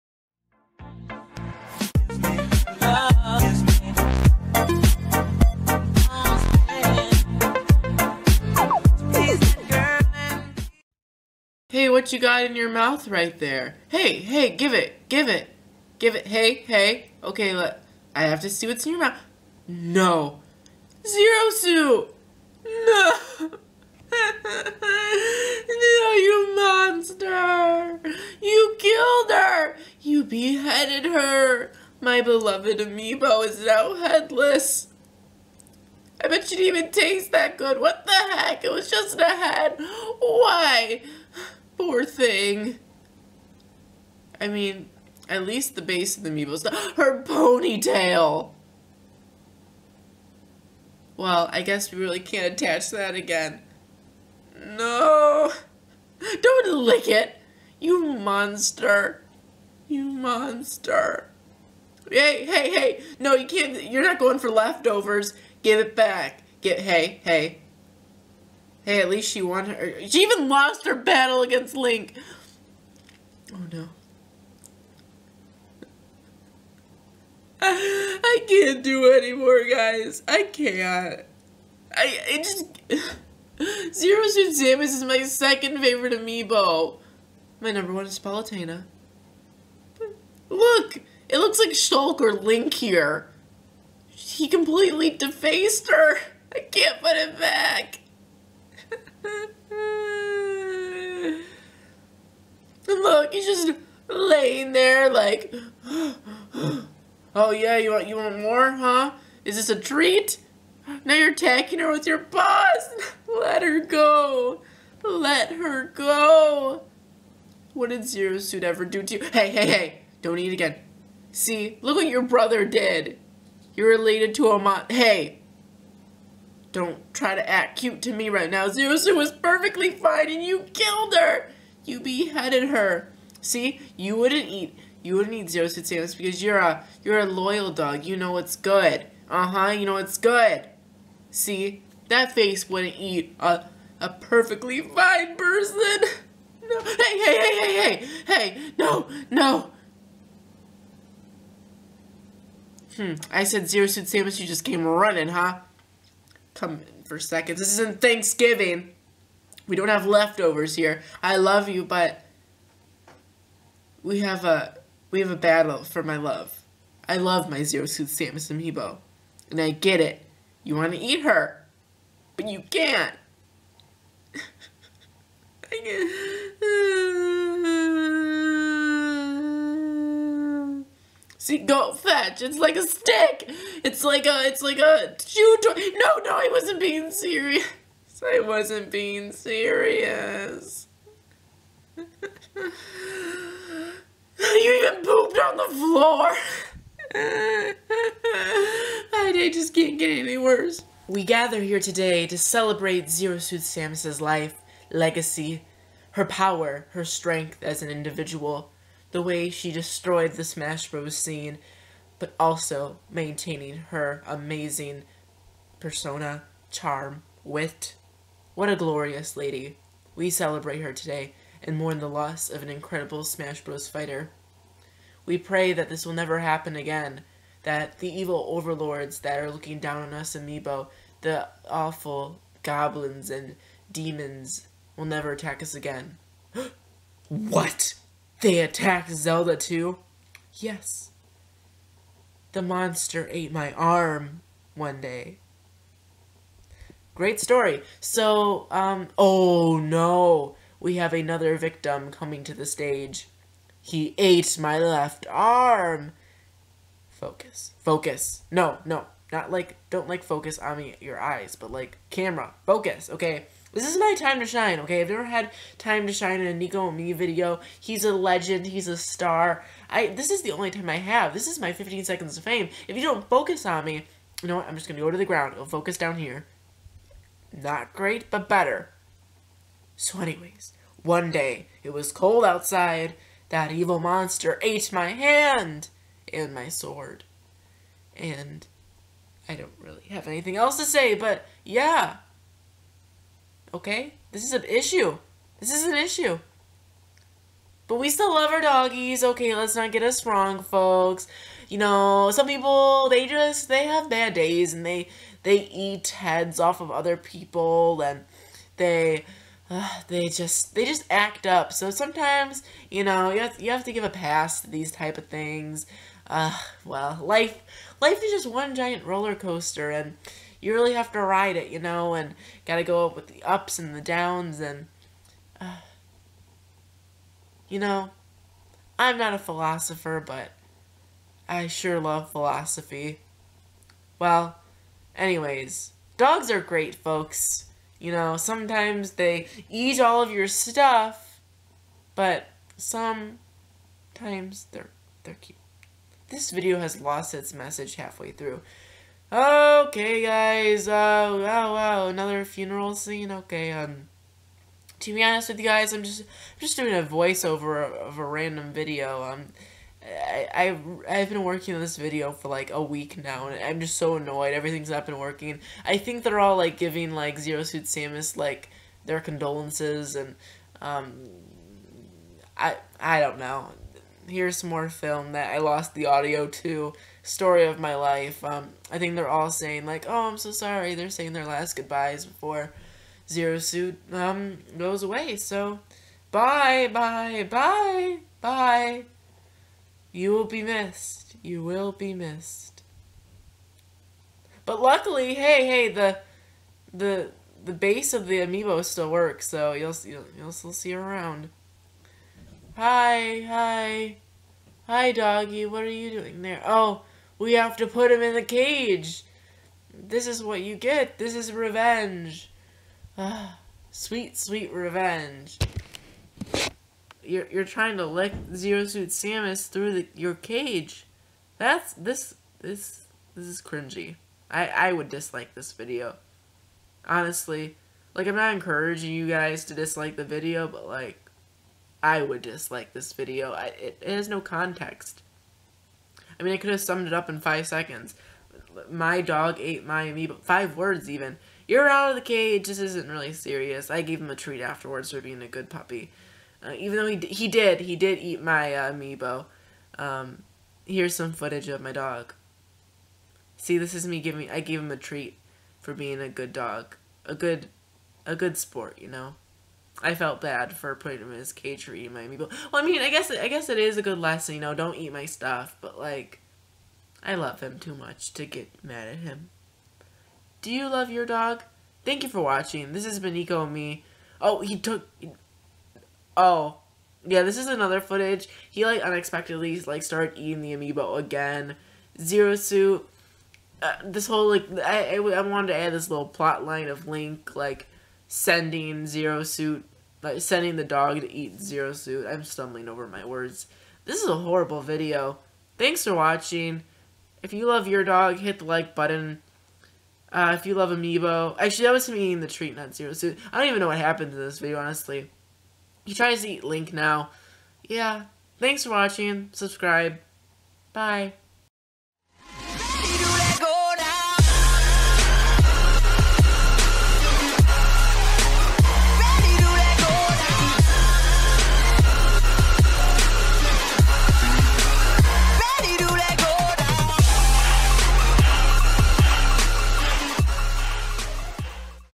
My Dog Ate My Amiibo
The sad, sad story about the time my dog ate my amiibo (with video of it happening)

R.I.P. Zero Suit Samus Amiibo